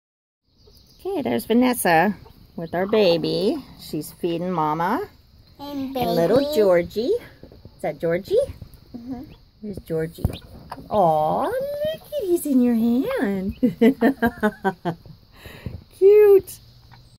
Our little Georgie
Our first baby chick hatched by one of my hens